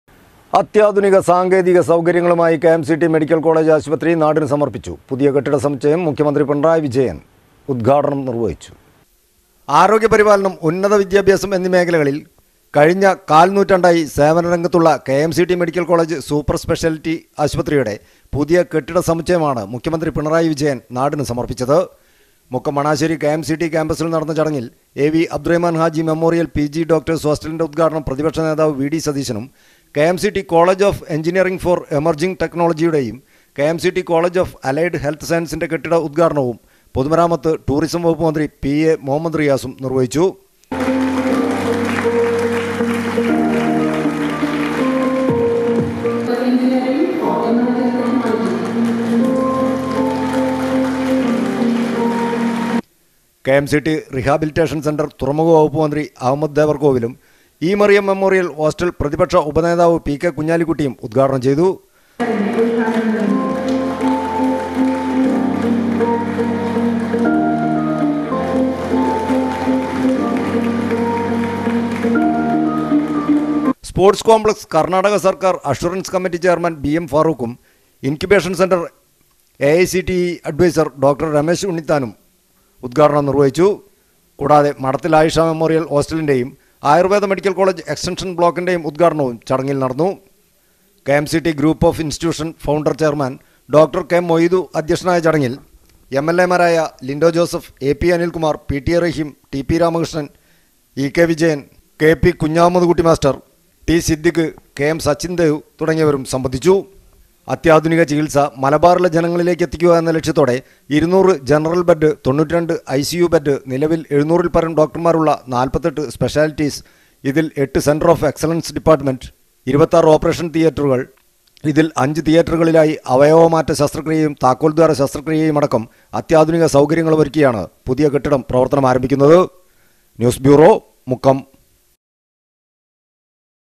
കെ.എം.സി.ടി മെഡിക്കൽ കോളേജ് ആശുപത്രി നാടിനു സമർപ്പിച്ചു.
അത്യാധുനിക സാങ്കേതിക സൗകര്യങ്ങളുമായി  കെ.എം.സി.ടി മെഡിക്കൽ കോളേജ് ആശുപത്രി നാടിനു സമർപ്പിച്ചു. പുതിയ കെട്ടിട സമുച്ചയം മുഖ്യമന്ത്രി പിണറായി വിജയൻ ആശുപത്രിയുടെ ഉത്ഘാടനകർമ്മം നിർവ്വഹിച്ചു.